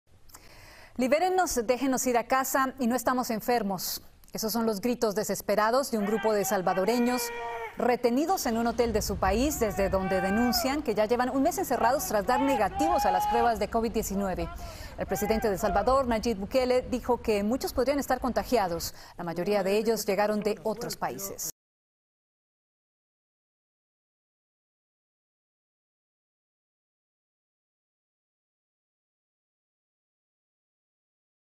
Salvadoreños piden ser liberados tras dar negativo a COVID-19 | Noticias Telemundo
Video oficial de Noticias Telemundo. "Libérenos, déjenos ir a casa y no estamos enfermos", este es el grito de un grupo de salvadoreños encerrados en un hotel en el cual aseguran llevan un mes retenidos luego de dar negativo al coronavirus.